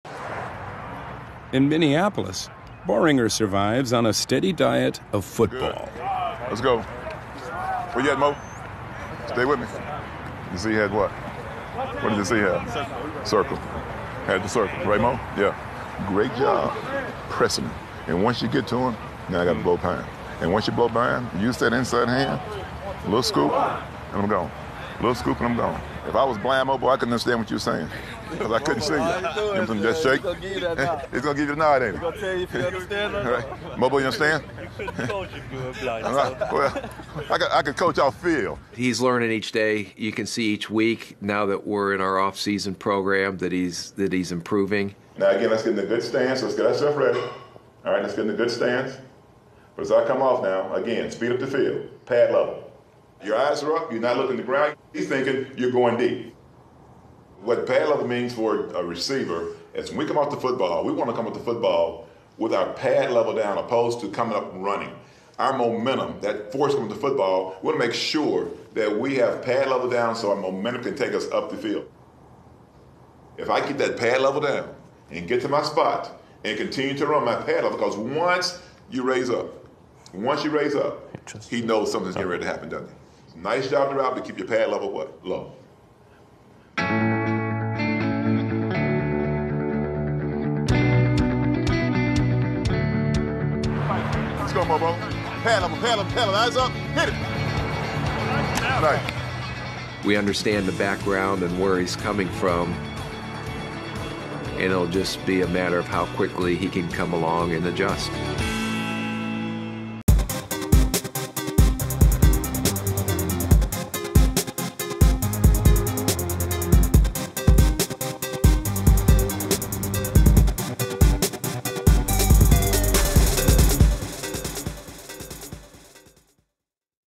Moritz Böhringer Behind the Scenes at Minicamp | NFL
Take a look at Moritz Böhringer "Behind the Scenes" during minicamp before his NFL rookie season with the Minnesota Vikings.